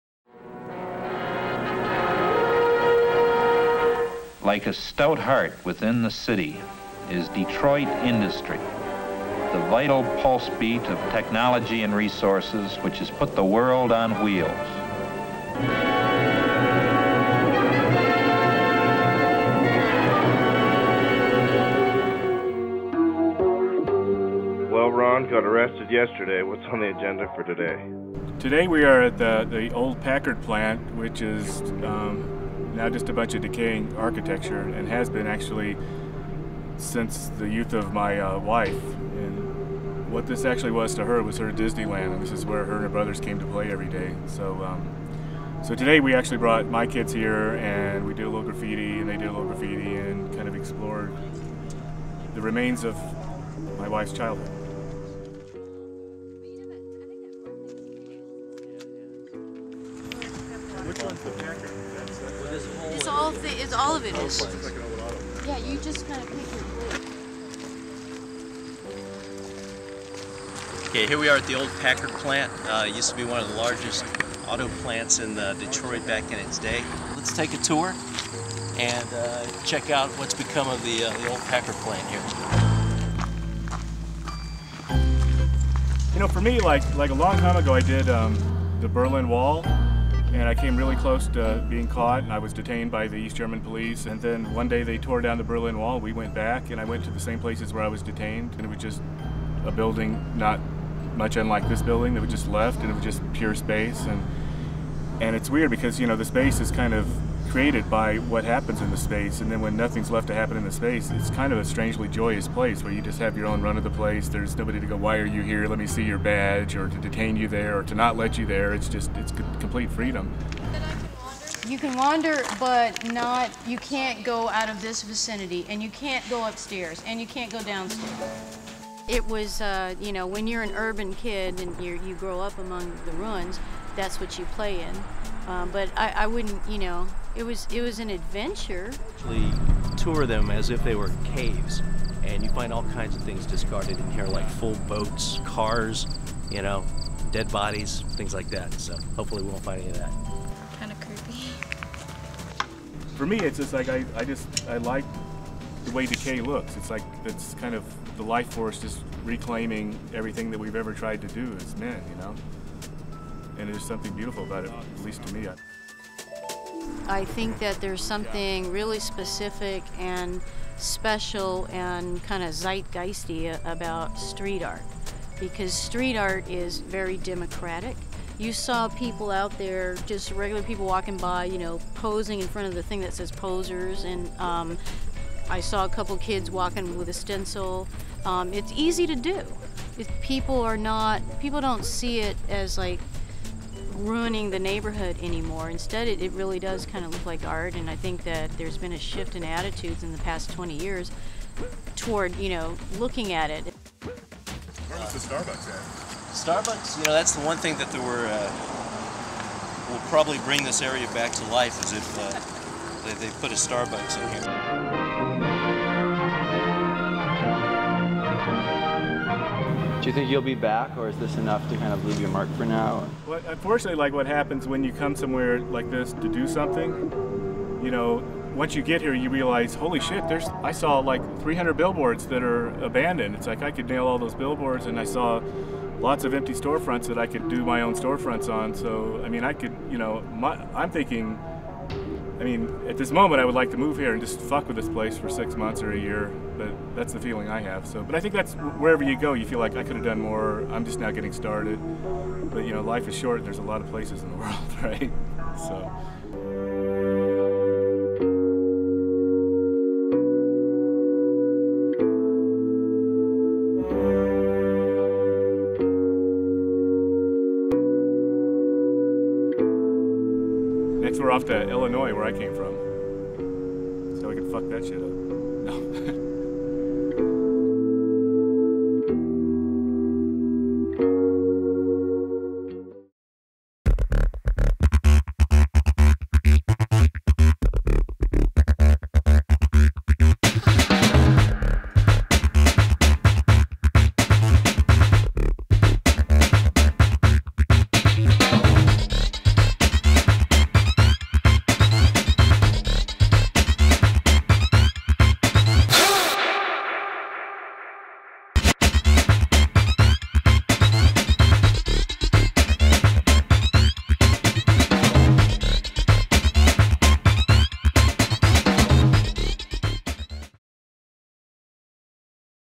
Ron English - The Detroit Project Part 3
Part 3:
After a day of wheat pasting, run-ins with the DPD and creating a giant mural, Ron and crew hit the now abandoned Packard Plant. We had a blast in this urban art playground. Check it out!

The Detroit Project:
After a lot of hard work and a summer of fun, TOTT Global, Popaganda and Ebaum's World present to you, "The Detroit Project - An English Family Vacation." We traveled from New York to Detroit documenting Ron English and the integration of his family into his art and pranks. This project was a great example of the collaboration between generations and artistic backgrounds.